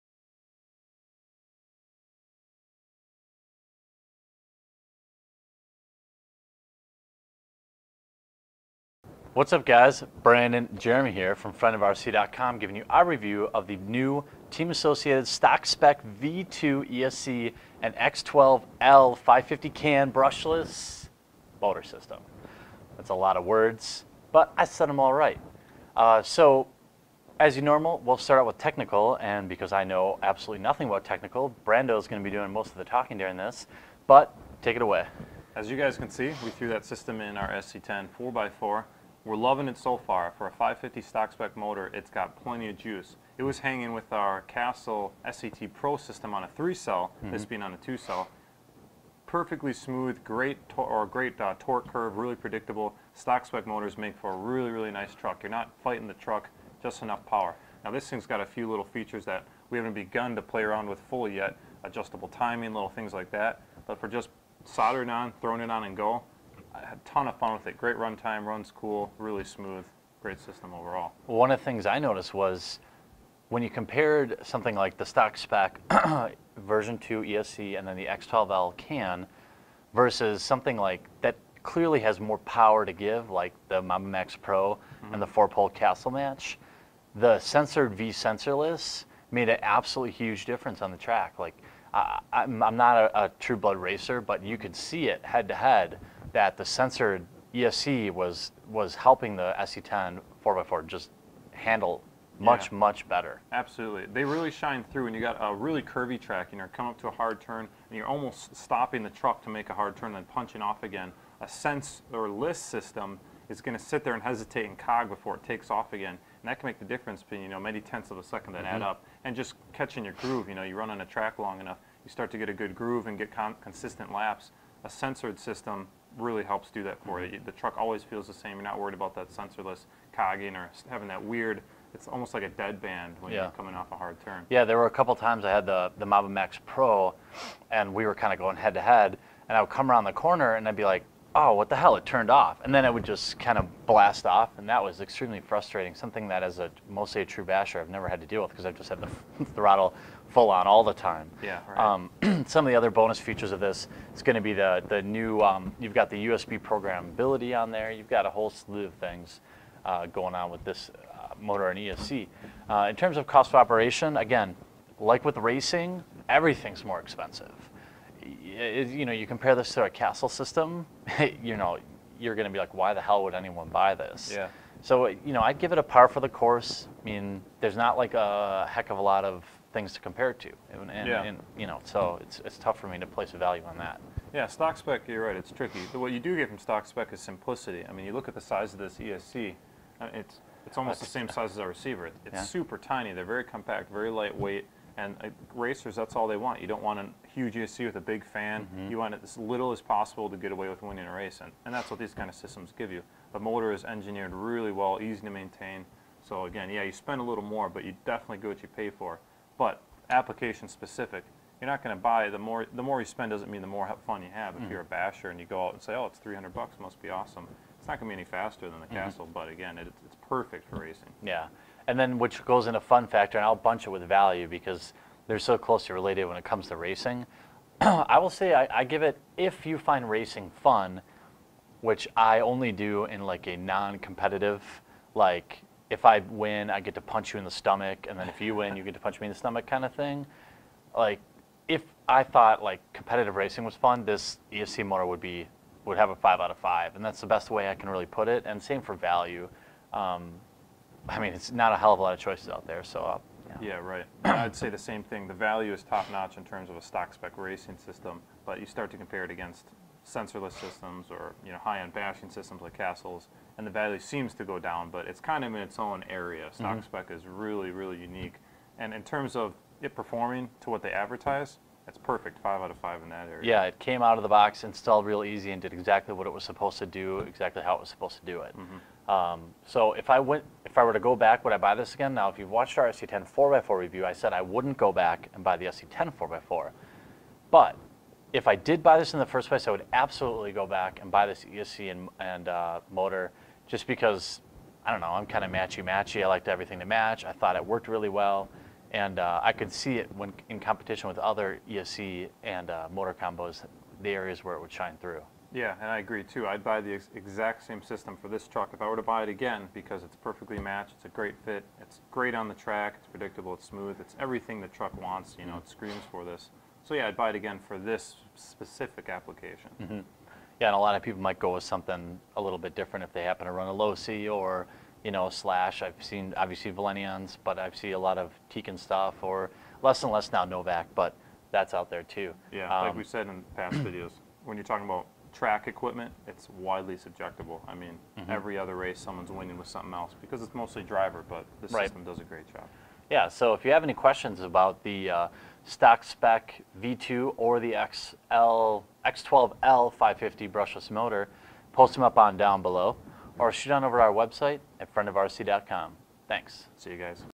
Team Associated XL12 550 Brushless Motor Review
We take a look at the brand new team associated XL12 550 Brushless motor with the stock spec version two speed controller by team associated.  This is the perfect setup for racing with a 4x4 electric rc short course truck

If you have any questions at all visit our forum
it's just much easier to manage, and there are more people there to answer questions